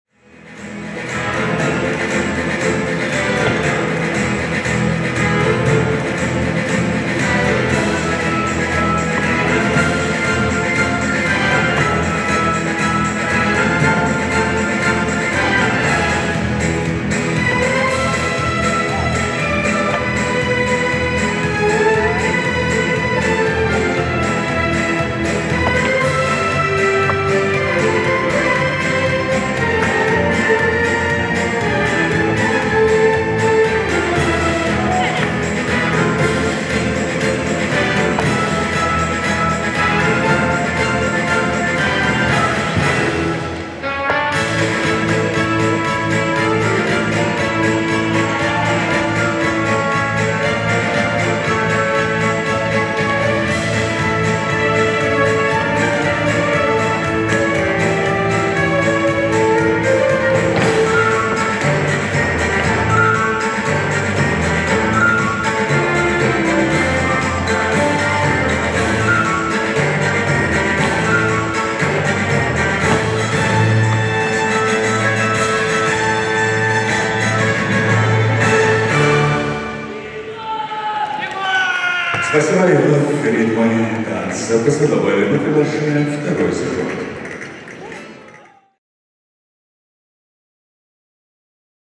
Gleb Bannikov - Ada Varstala FIN, Pasodoble, Imperia Cup 2019
Imperia Cup 2019, Moscow, Russia, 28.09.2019

1. Artur Balandin - Anna Salita GER
2. Semen Khrzhanovskiy - Elizaveta Lykhina RUS
3. LI Jiaming - LI Ziwei CHN
4. Dmitry Kulebakin - Daria Sviridenko RUS
5. Tomer Zveniatsky - Stefany Tzukerman ISR
6. Egor Kulikov - Maria Goroshko RUS

7. Daniil Ulanov - Kateryna Isakovych CYP
8. Petter Andreas Engan - Kine Marie Mardal NOR
9. Ayan Zhumatayev - Ekaterina Panteleeva KAZ
10. Gleb Bannikov - Ada Varstala FIN
11. - 12. Mikhail Efimovskikh - Asia Shamsueva RUS
11. - 12. Pavel Shchetinin - Anastasia Gorzhuy RUS